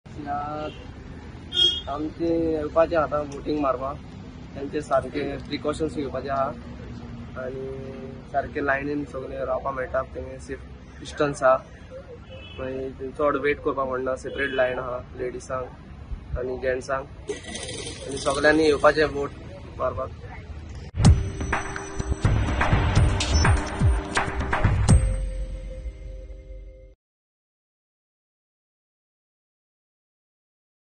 First time voter talks about his voting experience at AC No. 29, Curtorim-Goa
First time voter talks about his voting experience at AC No. 29, Curtorim-Goa State Agri MKtg Board, Arlem 6.